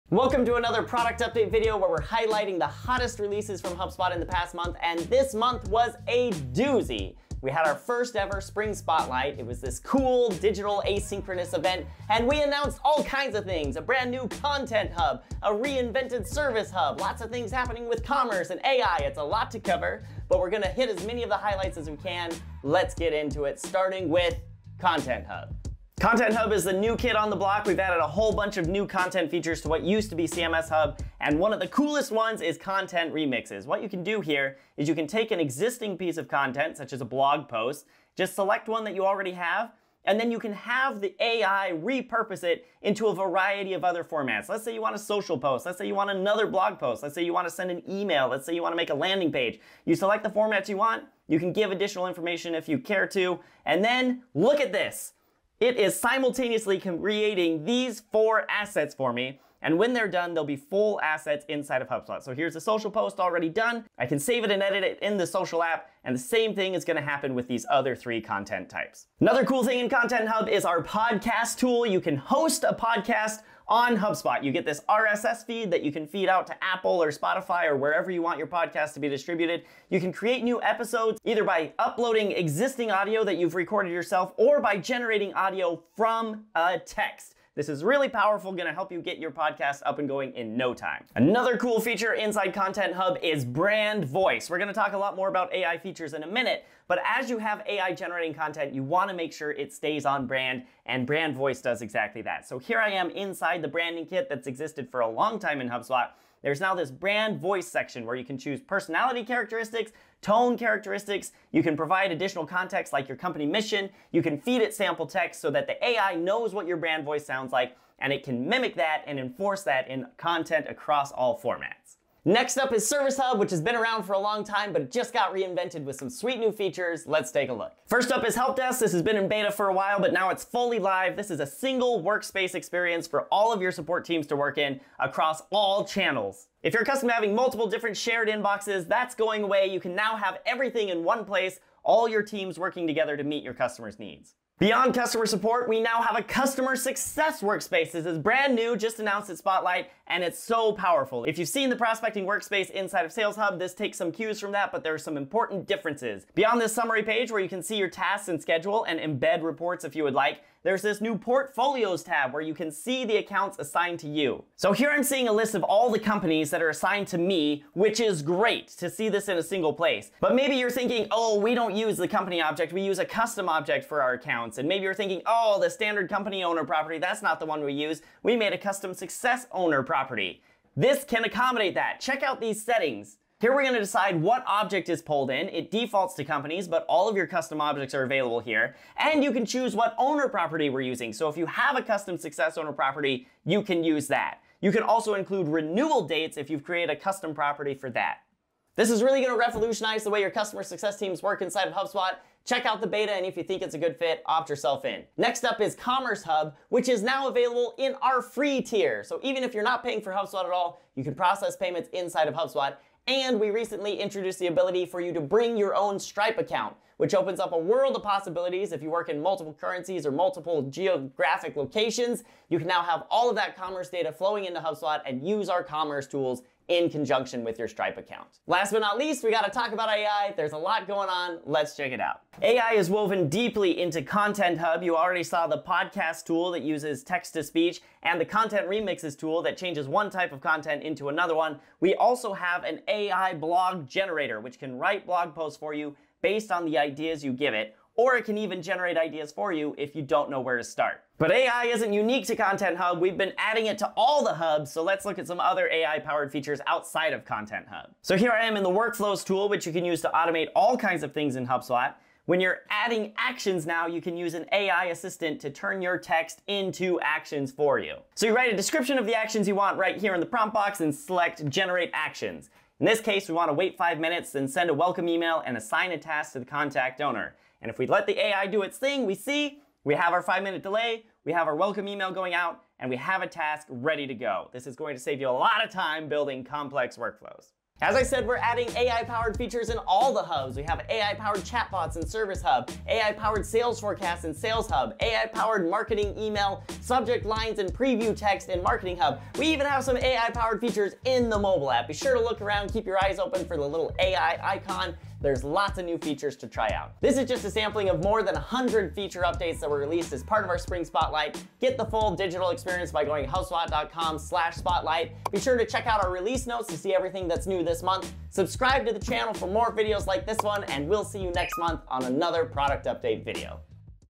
HubSpot Product Updates: Content Hub & More (5/2024)
Discover HubSpot's exciting new releases in our latest product update video! We dive into the innovative Content Hub, revamped Service Hub, and the latest in Commerce and AI. From AI-driven content remixes to a comprehensive customer success workspace, these features are set to revolutionize your business operations.

Timestamps:
0:00 Introduction to Spring Spotlight
0:23 Introduction to Content Hub
0:32 Content Remixes Feature
1:09 Podcast Tool in Content Hub
1:31 Brand Voice Feature
2:01 Introduction to Revamped Service Hub
2:08 Help Desk and Customer Success Workspace
3:26 Commerce Hub Updates
3:48 AI Features Across Hubs